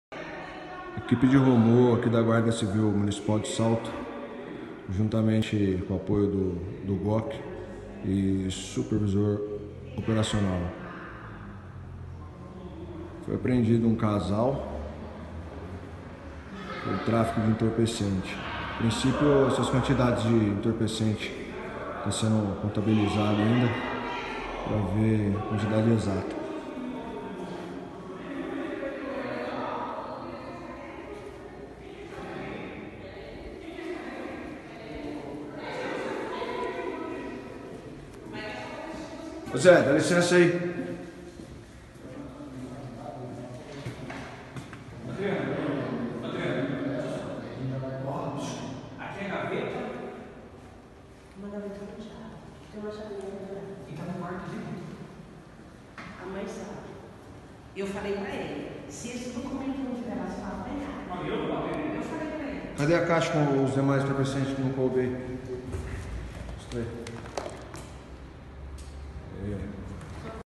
GCM de Salto dá duro golpe no tráfico com apreensão de muita cocaína
Denúncia levou GCM, com apoio da viatura do Grupo de Operação com Cães, a encontrar quase 5 mil porções de cocaína prontas para o consumo em casa do Jardim Santa Cruz